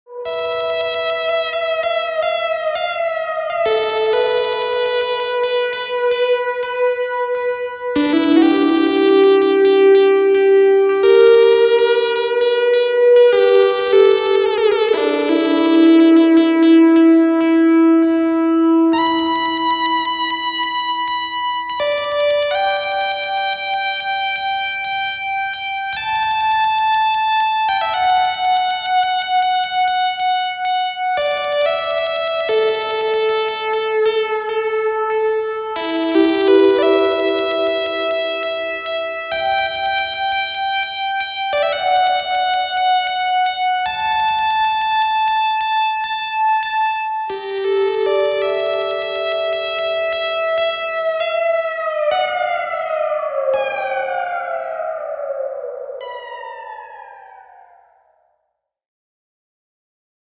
Sci-Fi Envelope-LFO Moog Patch | Sound Recipes #65
Trying to find new, interesting ways to patch your semi-modular synth? This recipe covers a quick and easy, 4-cable patch using the Envelope (+) out, the LFO Rate In & the Envelope Amount In.